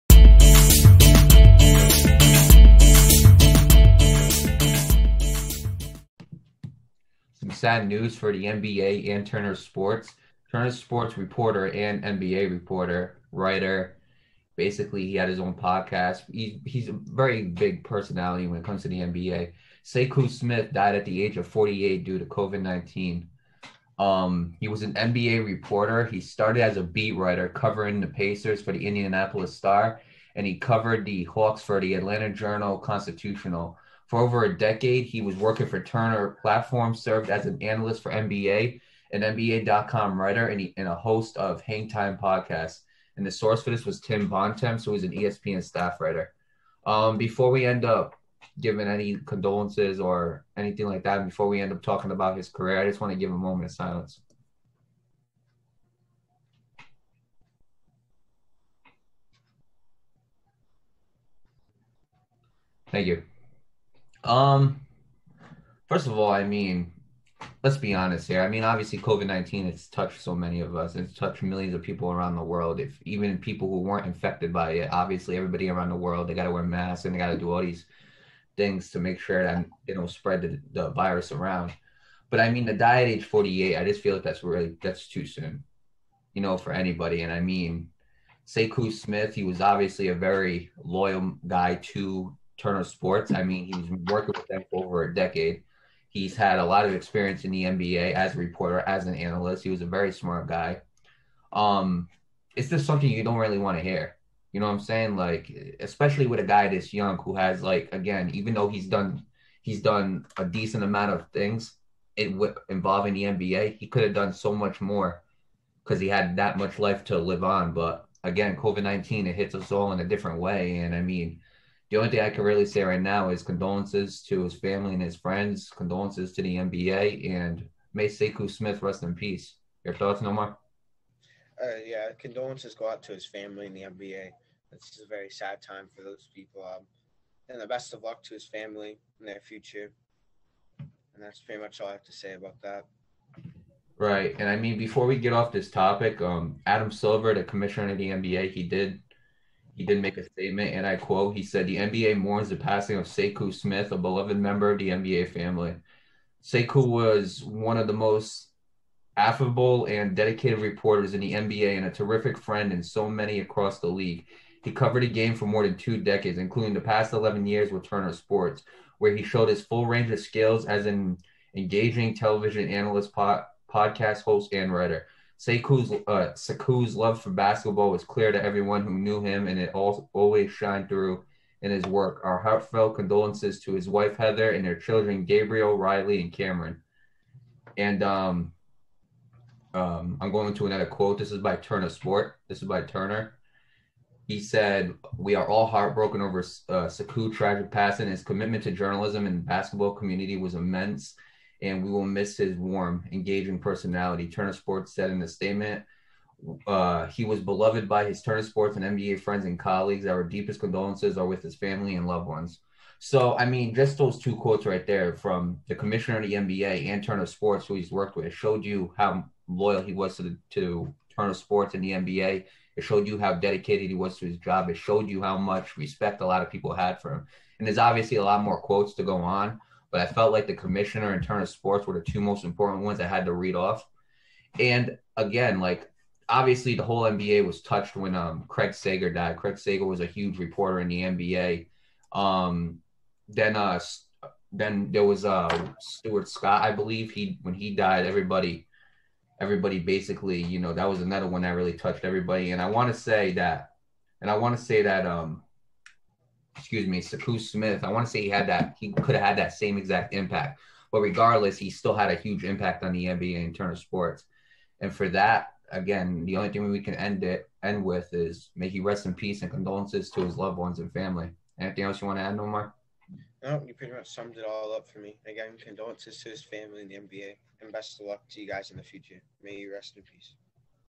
RIP Sekou Smith (January 28, 2021)
NBA reporter/analyst Sekou Smith has passed away at the age of 48 due to COVID-19. May his soul rest in piece.